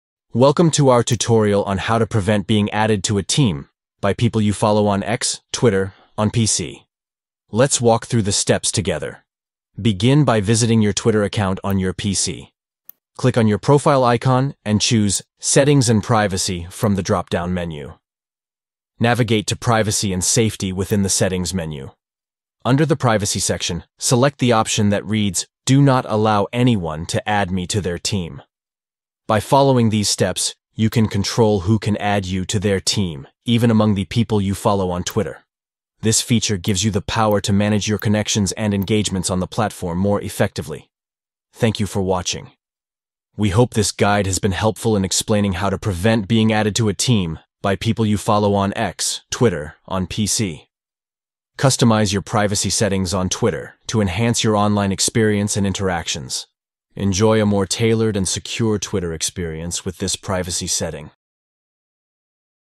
How To Prevent Adding To A Team From People You Follow On X (Twitter) On PC
Learn how to maintain control over your Twitter account by preventing people you follow from adding you to a team on PC. This tutorial video provides step-by-step instructions on adjusting your account settings to enhance your privacy and security on Twitter. Take charge of your online presence and ensure that only desired individuals can add you to their team with these simple steps.

Steps:

1. Visit Your account.
2. Click or tap on your profile icon, then select Settings and Privacy.
3. Next, select Privacy and Safety.
4. Finally, under Privacy, choose Do not allow anyone to add me to their team.

By following the steps outlined in this tutorial, you can confidently prevent people you follow on Twitter from adding you to their team on your PC. Take charge of your account's privacy and security settings to ensure that your Twitter experience aligns with your preferences. Empower yourself with the knowledge to control who can add you to their team and enhance your online safety.